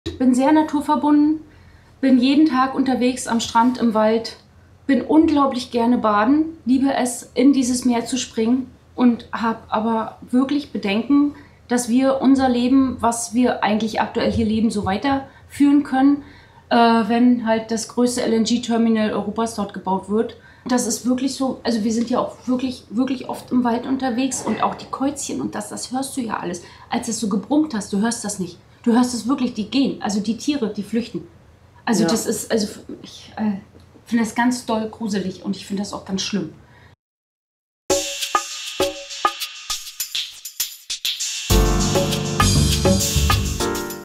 Sophia Koßmann, Restaurantbesitzerin Rüganer:innen über das geplante LNG Terminal vor Rügen
Nur gemeinsam können wir was erreichen! Unsere Einwohner sind sich einig - sie sagen nein zum geplanten LNG-Terminal und zeigen sich entschlossen im Kampf für ihre Insel. Bitte helft auch ihr mit und unterzeichnet die Bundestags-Petition.
Jede Stimme zählt!